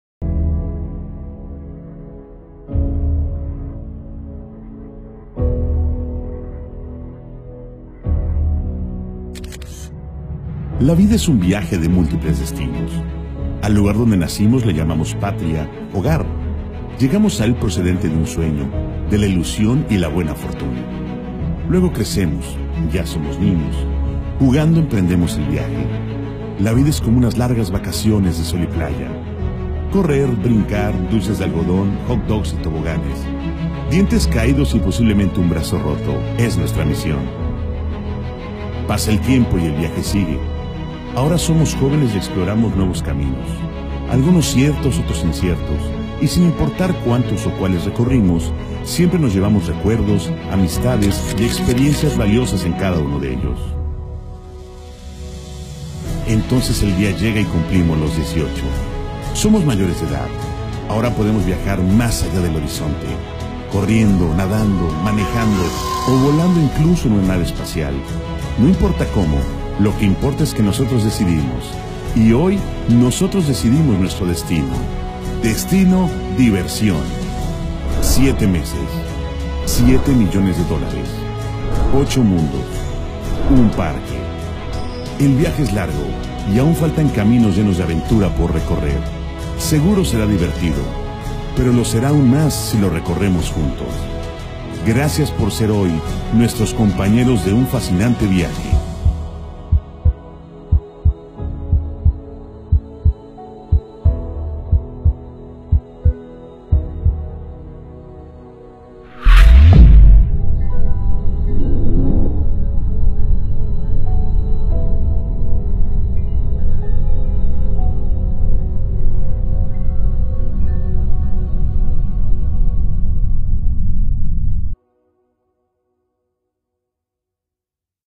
Wet’n Wild Cancún se convierte en "Aventura Park" / 2016
Wet’n Wild Cancún es uno de los parques acuáticos más importantes de México que se renueva fuerte tras una inversión de 7 millones de dolares para convertirse en AVENTURA PARK, el nuevo parque que mantendrá la esencia del parque acuático agregando más atractivos a la propuesta anterior. Apertura al publico: 1° de Marzo 2016.